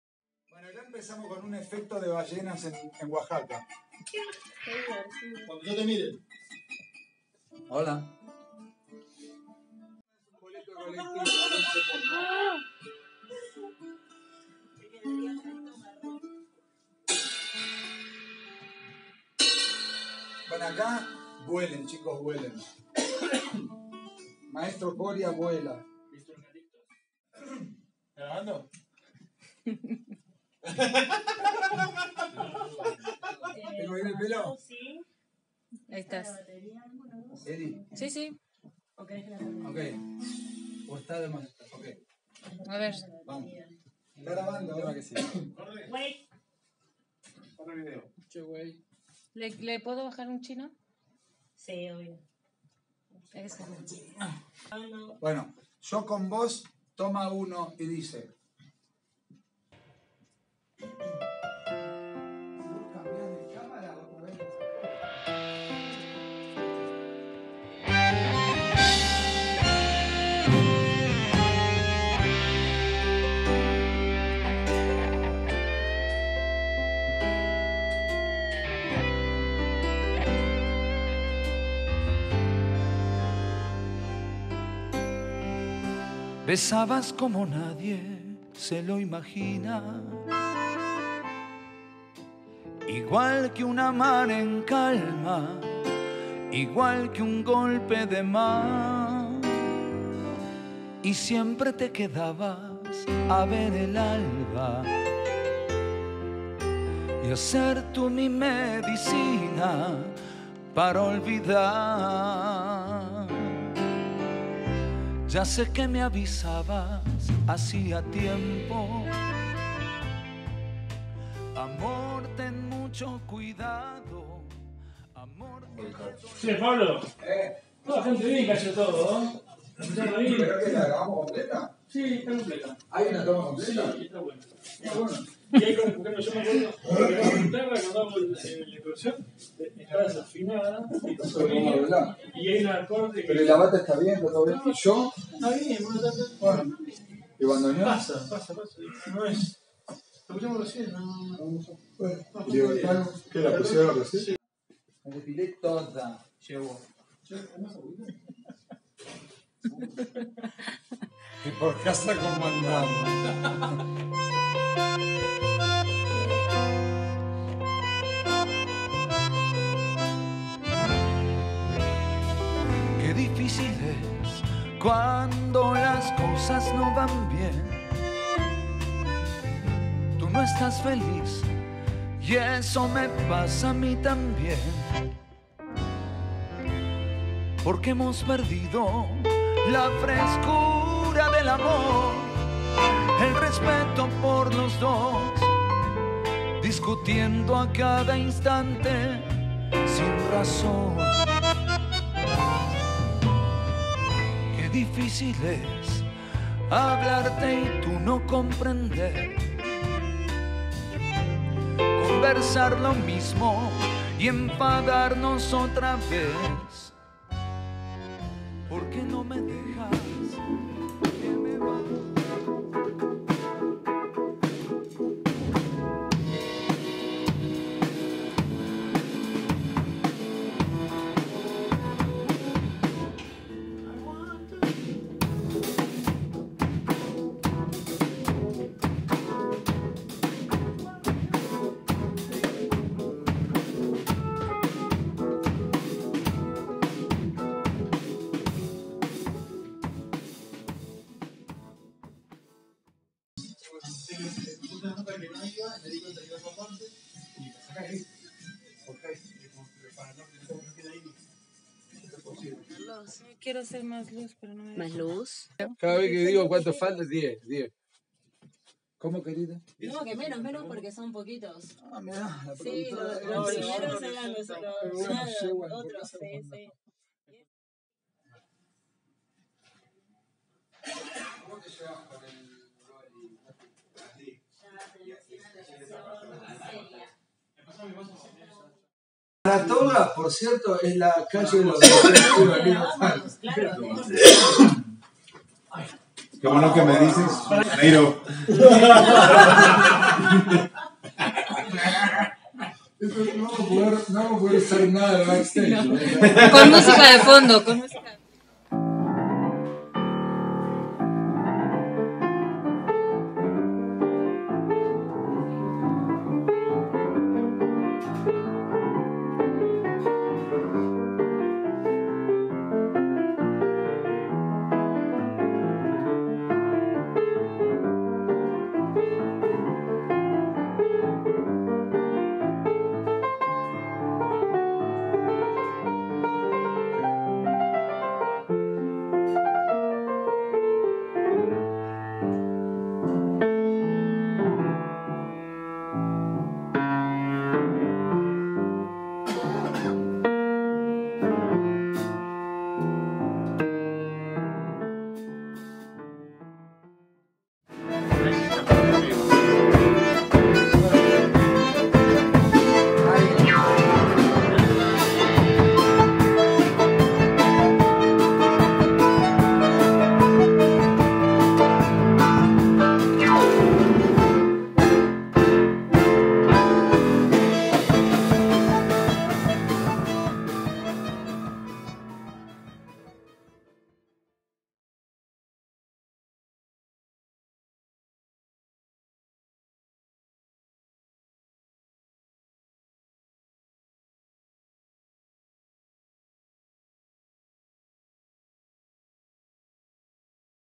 Backstage Pablo Ahmad (Live Sessions Fonobox 2019)
The making of Pablo Ahmad in Fonobox Live Session 2019.

El detrás de camaras de las grabaciones en vivo realizadas por Pablo Ahmad y su grupo de Tango Rock en los estudios FONOBOX en 2019 en la Ciudad de México.

Pablo Ahmad es músico, cantante, compositor y productor argentino radicado en México.

Staff de Producción:

Hernán Fariña, Nacho Auditore, Elisa Parra y Natalia taramasco.

Ingeniero de grabación:

Jeronimo Gorraez

Edición de video:

Hernán Fariña

Músicos:

Juan Manuel Rodrígez (Batería)

Cesar Coria (Bajo)

Alejandro Gómez (Teclados)

Guitarra y dirección (Damián Tuso)